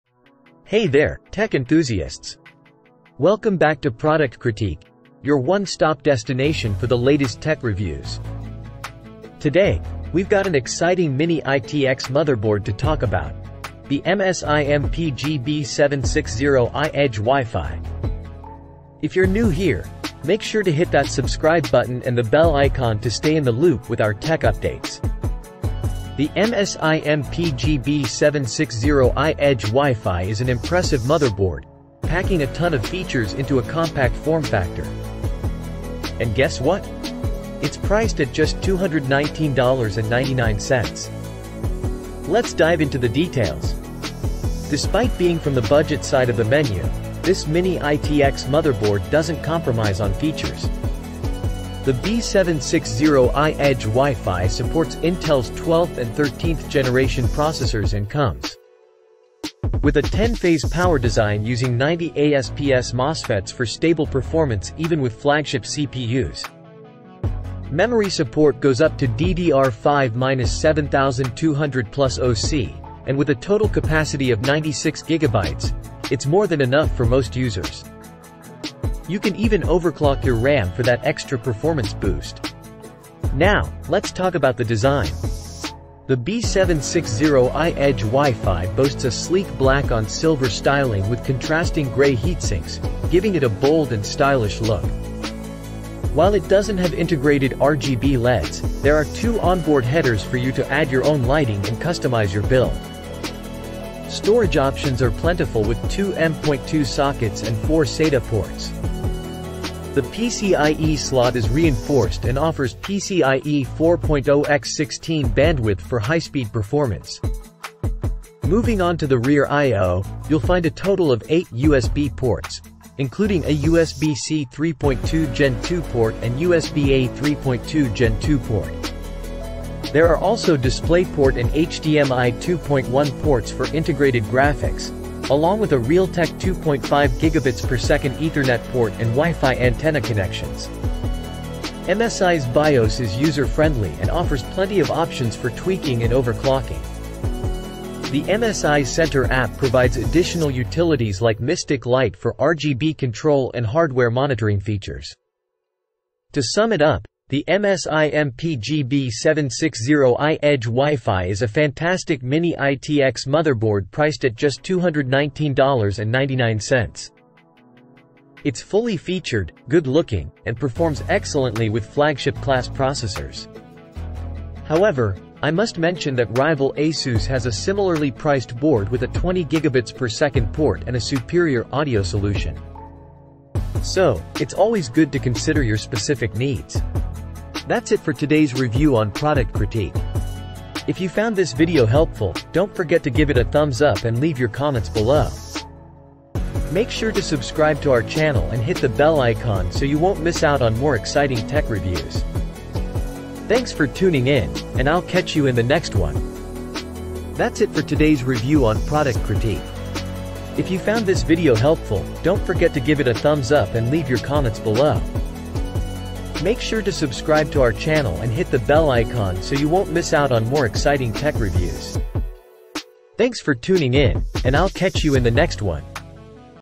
MSI MPG B760I Edge Wi-Fi: Unleashing Motherboard Excellence!
Amazon Product Name : MSI MPG B760I Edge Wi-Fi

Welcome to our MSI MPG B760I Edge Wi-Fi Review! In this video, we'll be diving into the features and performance of MSI's compact B760I Edge Wi-Fi motherboard designed for 11th Gen Intel Core processors. Despite its mini-ITX form factor, the MPG B760I Edge Wi-Fi offers a wealth of cutting-edge features, including PCIe 4.0 support, robust power delivery, and advanced connectivity options. Join us as we explore its gaming-oriented features, like support for high-speed memory and lightning-fast Wi-Fi 6E. We'll also take a closer look at its sleek design and customizable RGB lighting options. If you're in the market for a high-performance ITX motherboard that doesn't compromise on features, this review will help you decide if the MSI MPG B760I Edge Wi-Fi is the perfect fit for your next compact gaming rig or workstation.